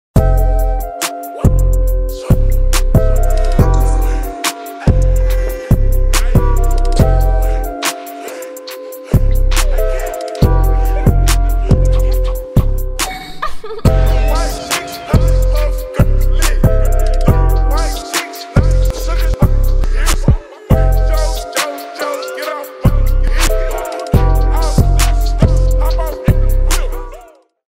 Brandon Shaw @ Elk City Motocross (OSCS Rd. 1 - 2020)
Brandon sending all the triples out at Elk City Mx